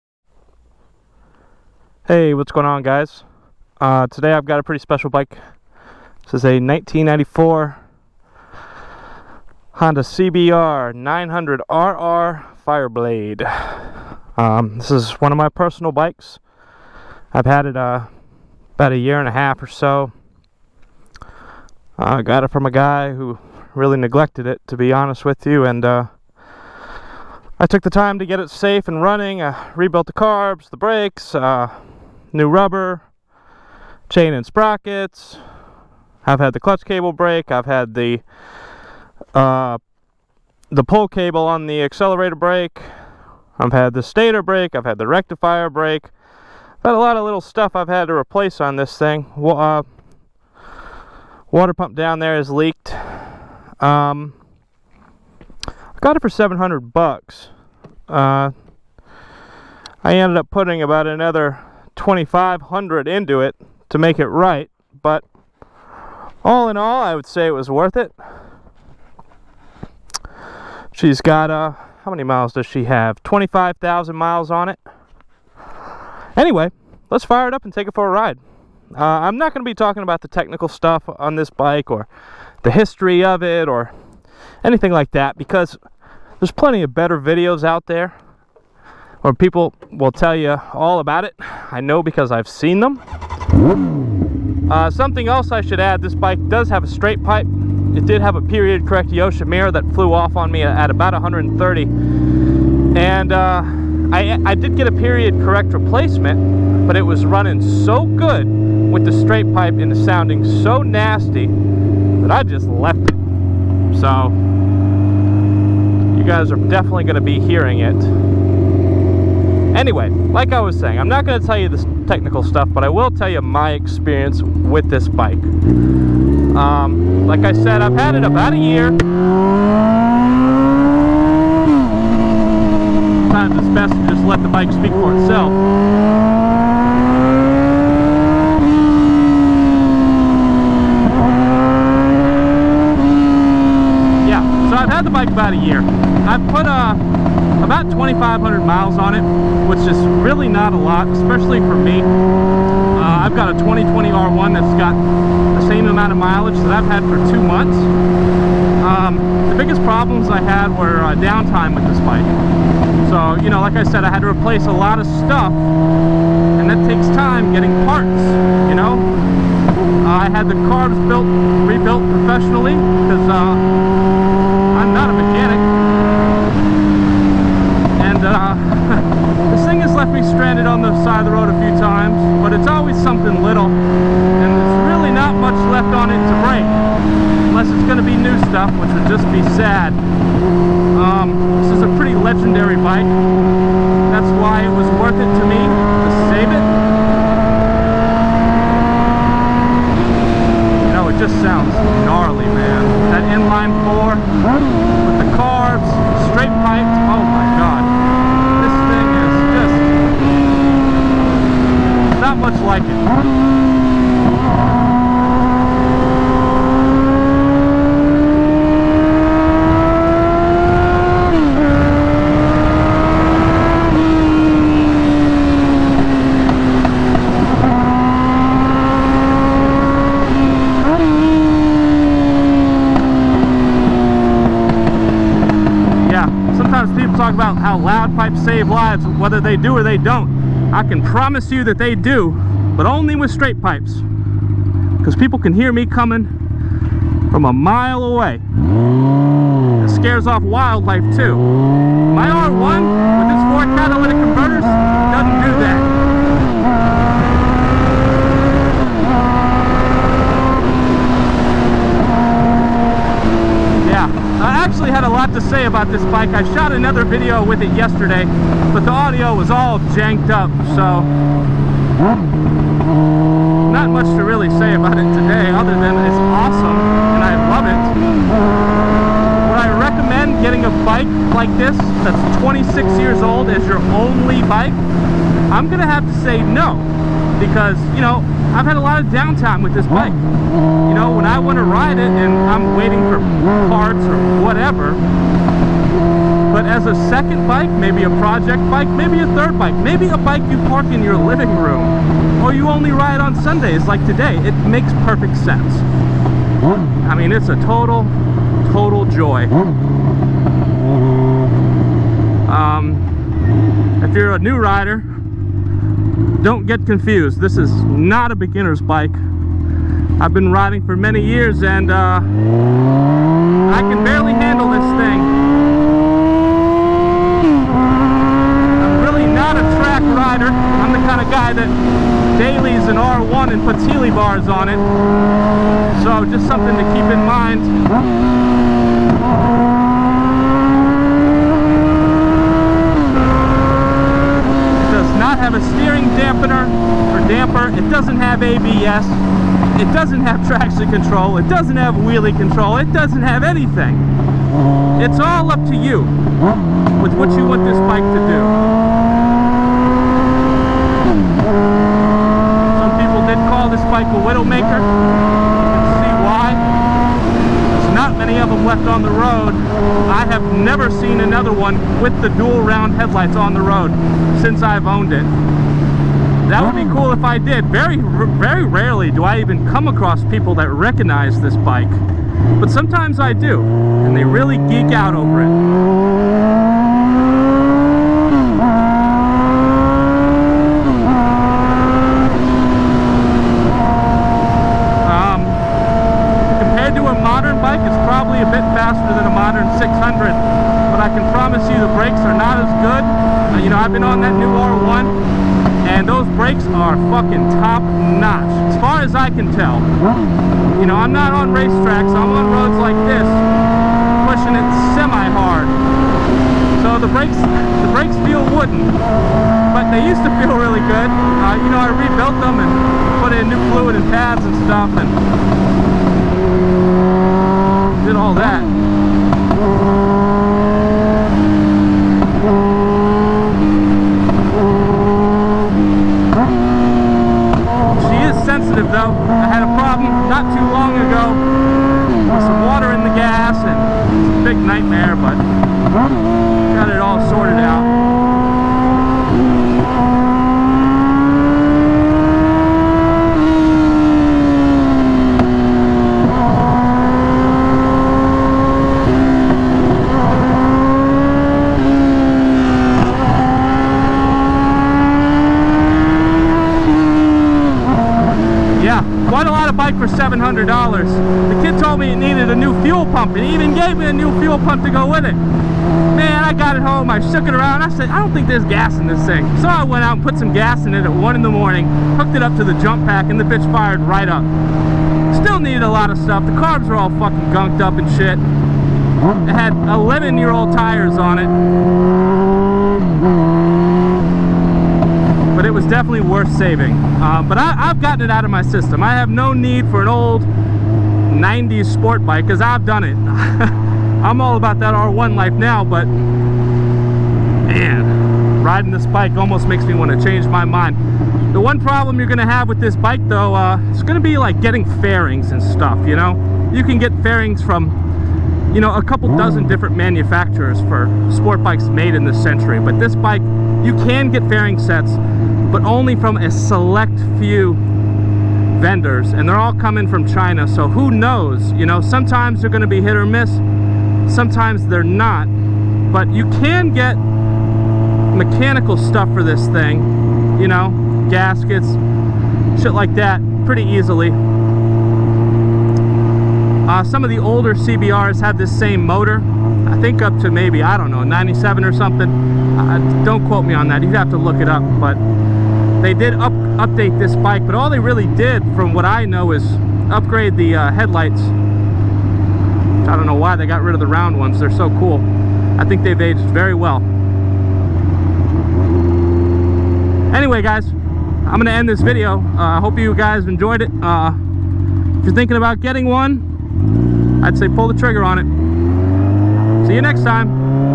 1994 Honda CBR 900RR Fireblade / Ride and Review
Just sharing my thoughts on a bike of mine. Low key trying a new mic set up too. If you’re into this kind of thing, enjoy.